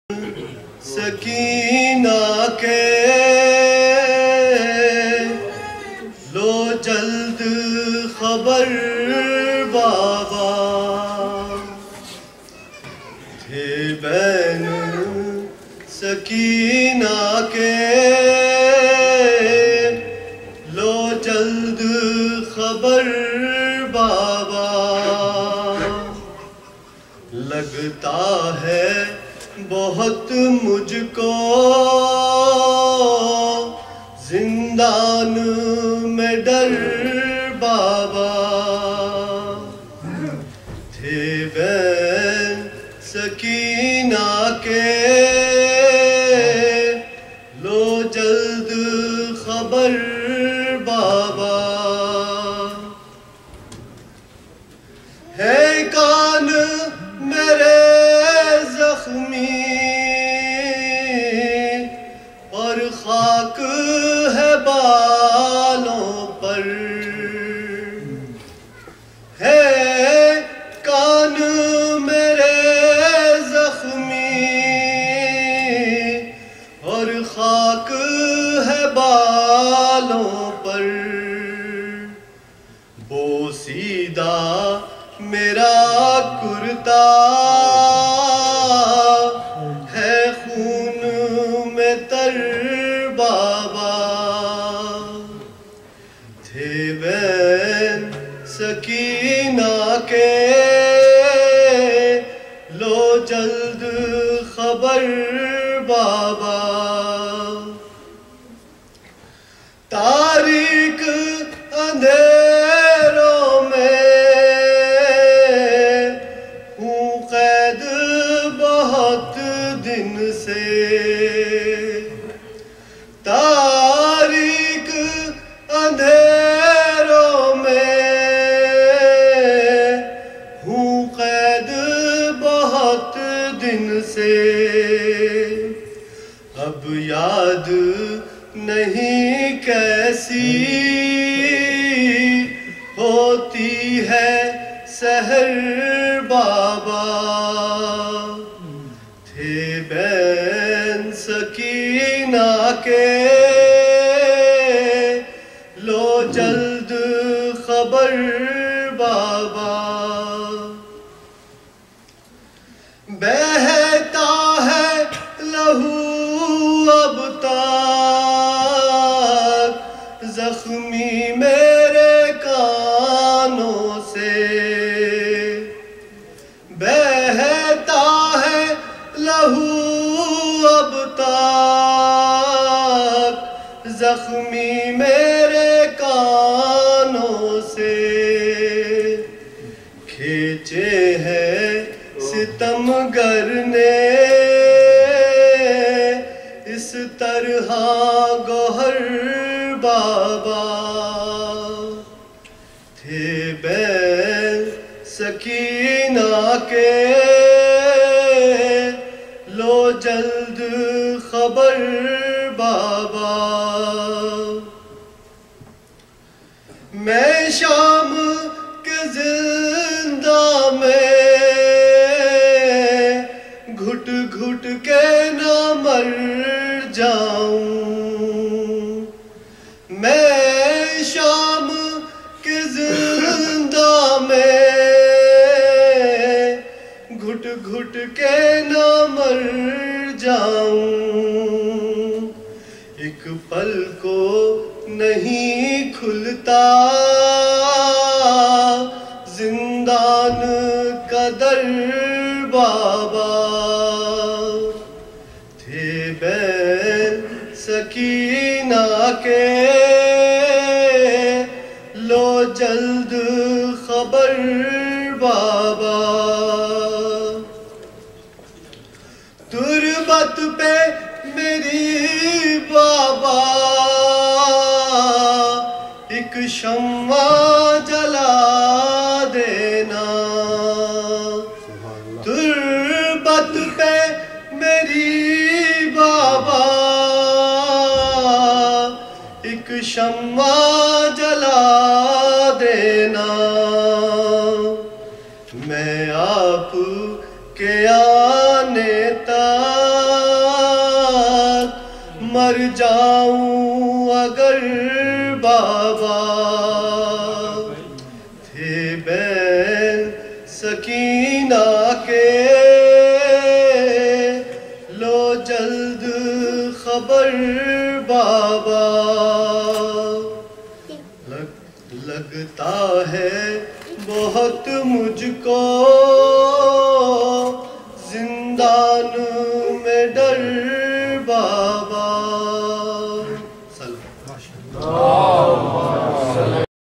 17 - Marsiya by Hassan Zaidi - Muharram 1434 (2012)
Shahe Najaf Islamic Center of Washington DC, Virginia, USA.

Copyright: Shahe Najaf Islamic Center of Washington DC